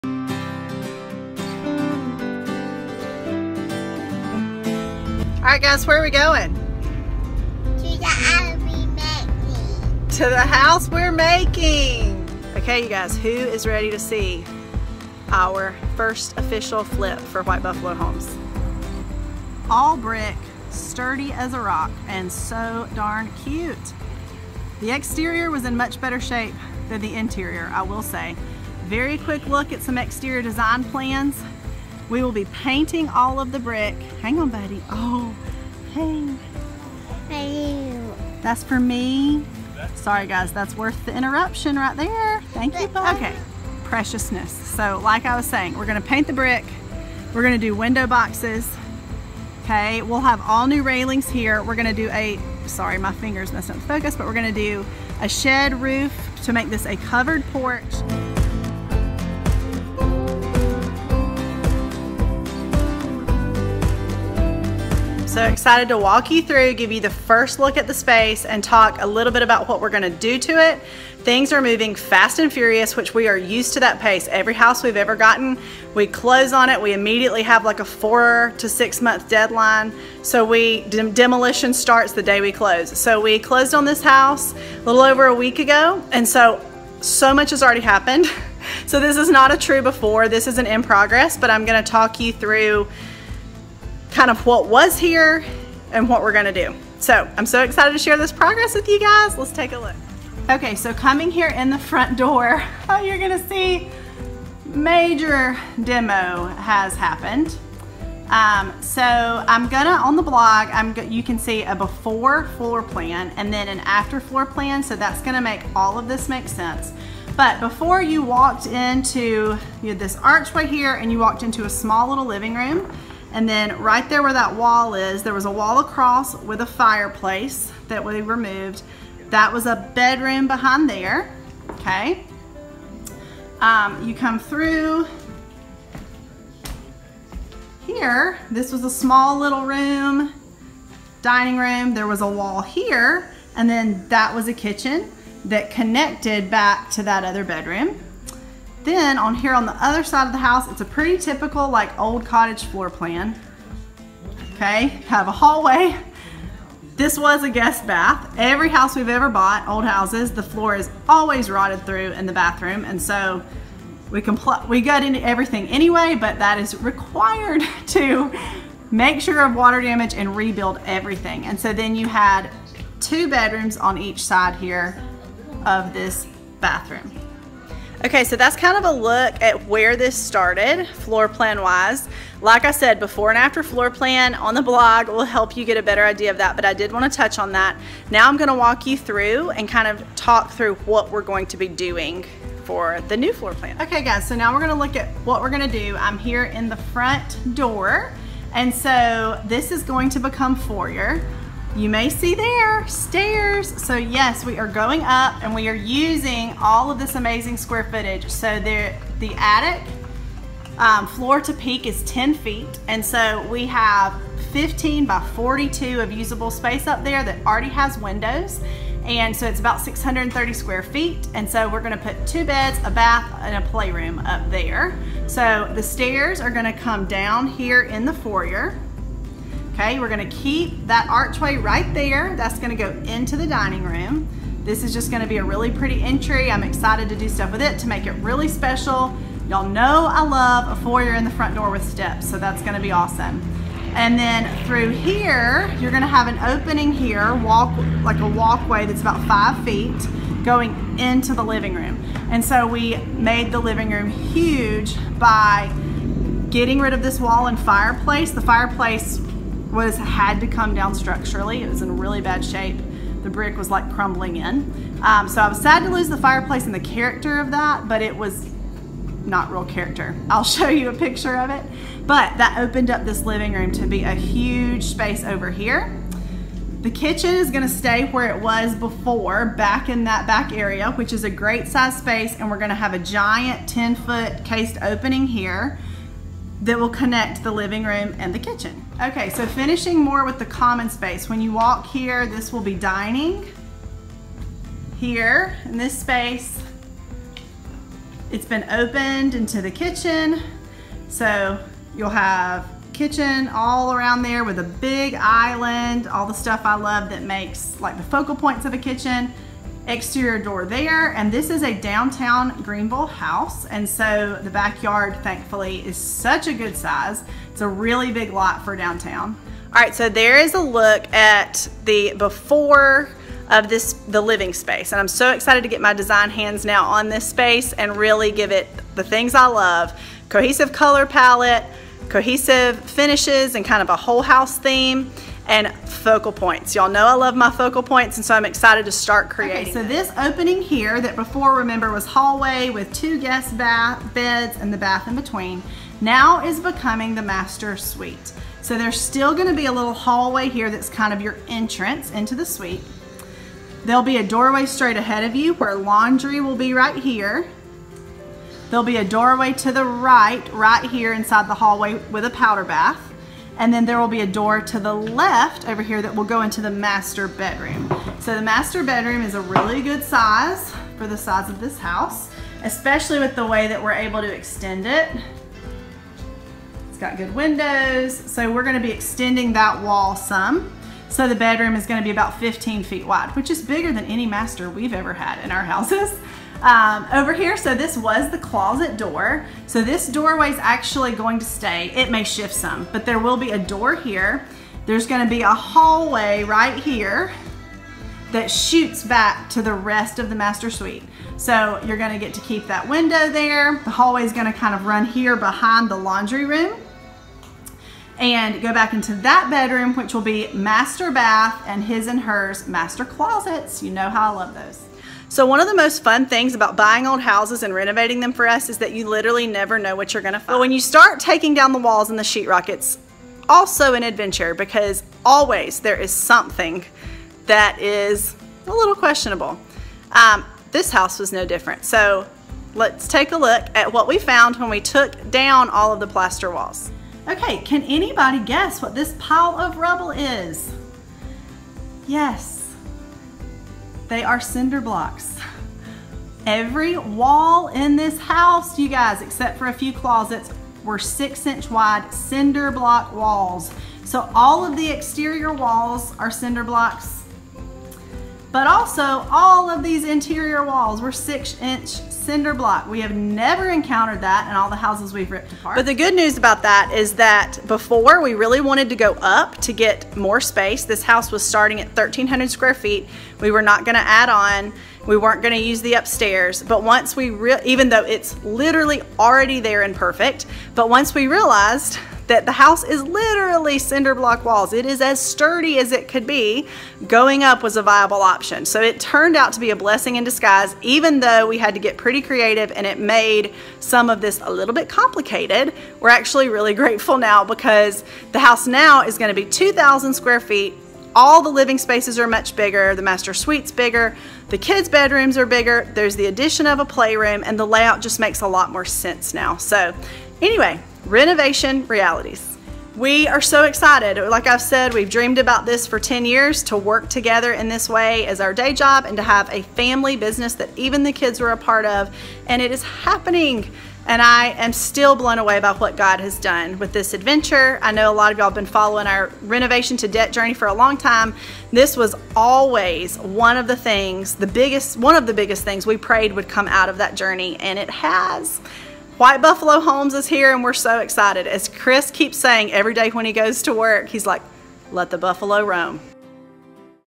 White Buffalo Homes First Flip
While we’ve flipped many houses over the years, this is the first property we’re officially flipping as White Buffalo Homes! I’m so excited to give you guys a glimpse of where this house is starting and what our big plans are! I’m so excited for the vision we have for this space, it’s going to be special!